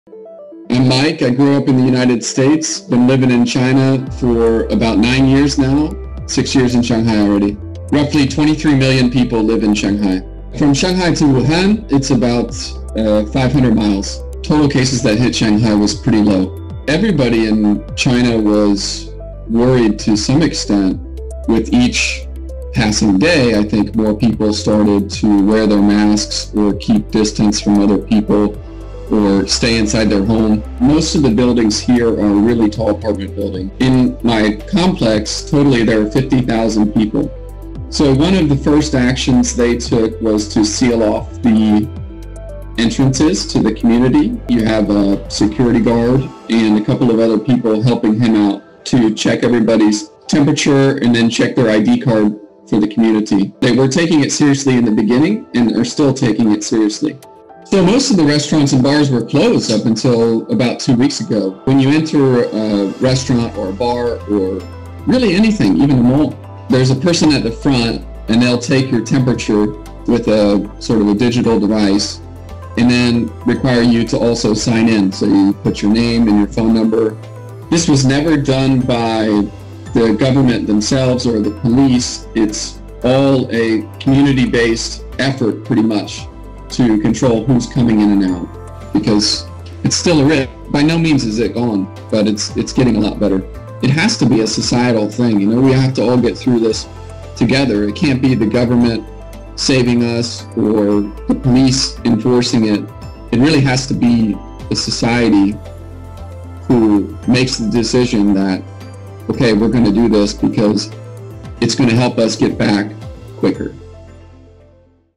An American's Experience in China During COVID-19 | WebMD
"It really has to be a society who makes the decision that, ok, we're going to do this, because it's going to help us get back quicker." One American details his experience living in China during the COVID-19 outbreak.